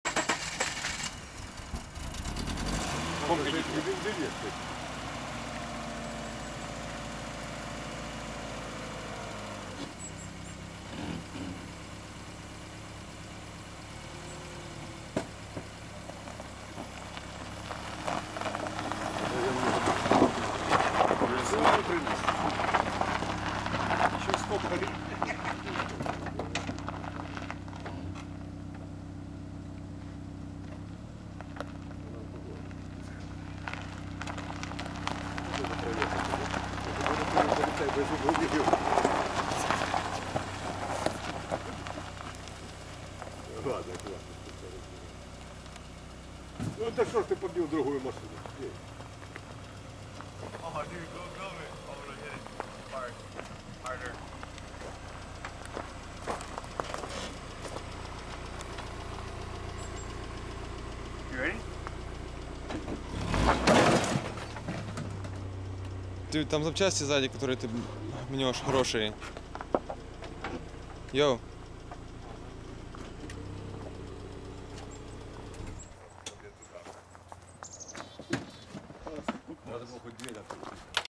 Subaru impreza, Yes it drives!
Bet you haven't seen half a car driving before. This is a scrap car we originally planned to make a dune buggy out of, but then we got a different suby toy that needed an engine so this is right before we pulled it out.